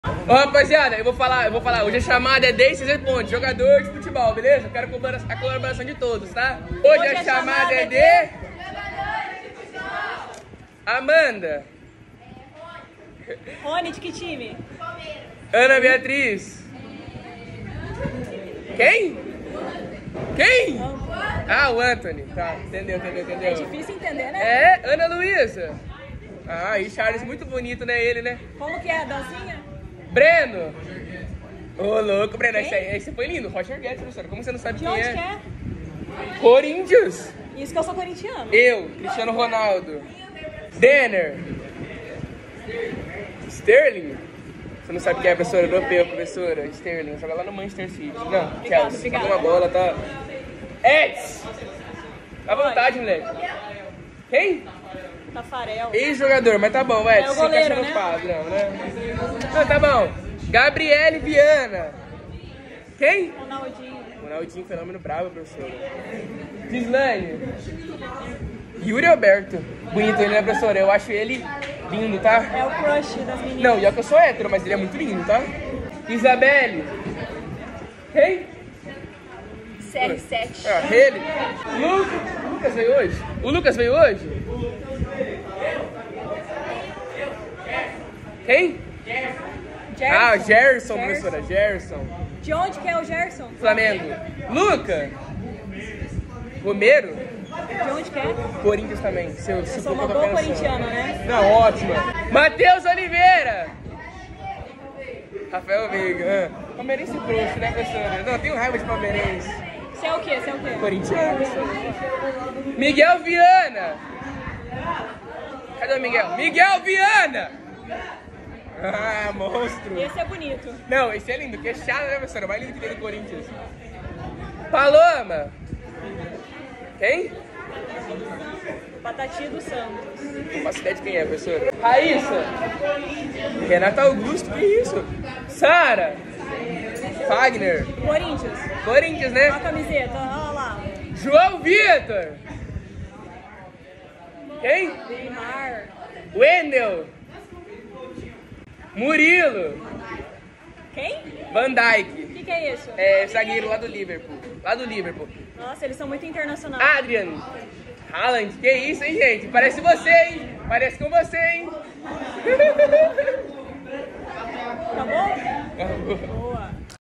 Hoje a chamada é de: JOGADORES DE FUTEBOL! Com participação do Cauã 😅
✨Rotina de uma prof. do Ensino médio. Ensinando com amor e diversão!